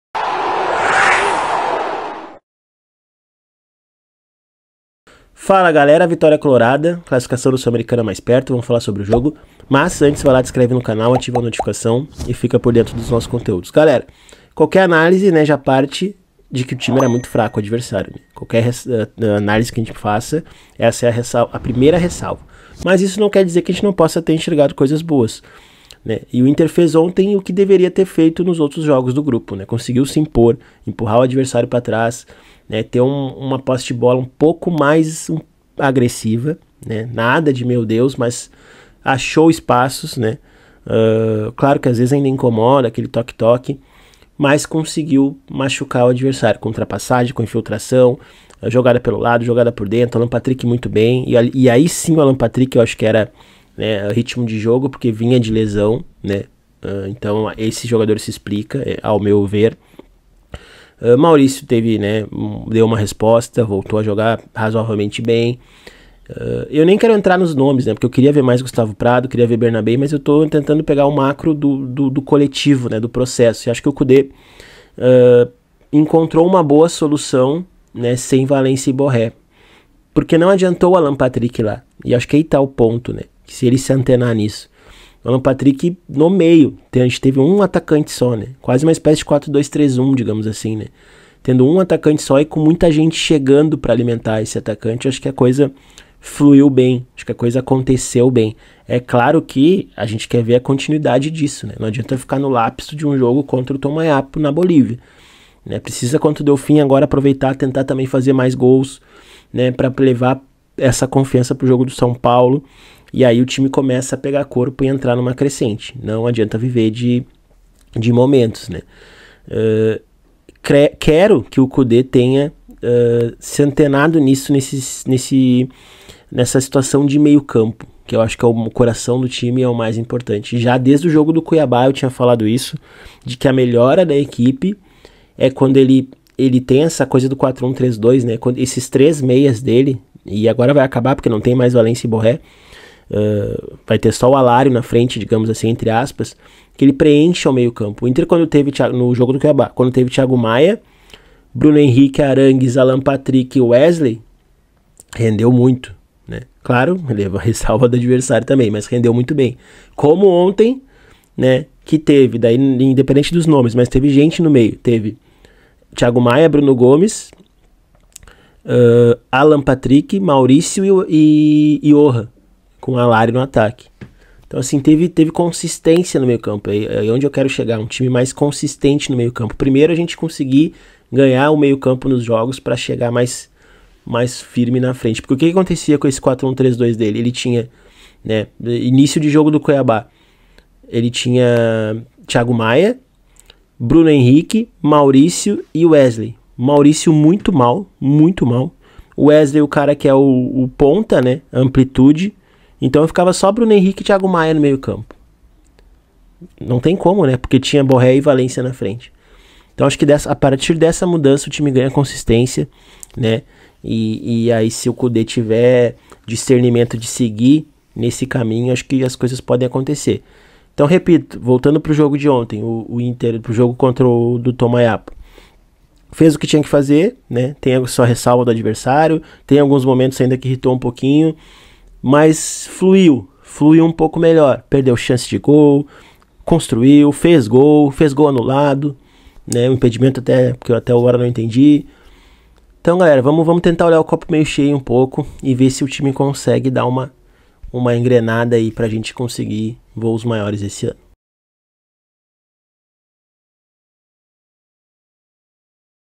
INTER 2x0 TOMAYAPO | ANALISE TÁTICA
Análise de INTER 2x0 Tomayapo. Vitória na sulamericana. Partida mais fluida, com o time conseguindo se impor contra um adversário fraco. Destaque para Alan Patrick. Maurício , Hyoran, Bruno Gomes e Fernando tambem fizeram bom jogo.

Já fez tua aposta hoje???